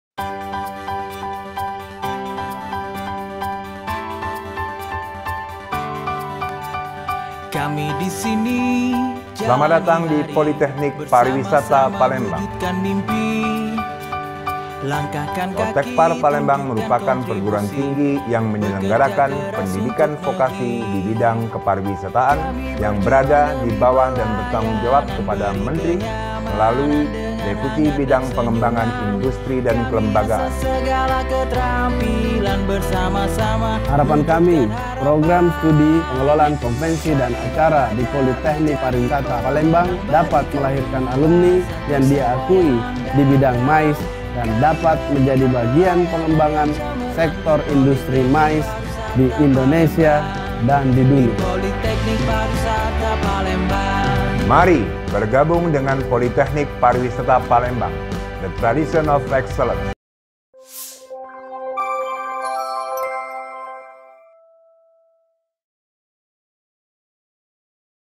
Program Studi Pengelolaan Konvensi dan Acara
Salah satu Program Studi unggulan di Politeknik Pariwisata palembang yaitu Prodi PKA, memiliki kompetensi untuk menciptakan lulusan profesional di tingkat managerial dalam MICE: Meeting, Incentive, Confrence and Exhibition (Pertemuan, Insentif, Konvensi dan Pameran).
Kompetensi Lulusan
Mampu membuat konsep, melaksanakan dan evaluasi konvensi atau acara, menghitung kebutuhan anggaran, memasarkan atau mempromosikan kegiatan, melakukan negosiasi dengan para penyedia kebutuhan konvensi dan acara, serta mendirikan usaha dibidang konvensi dan acara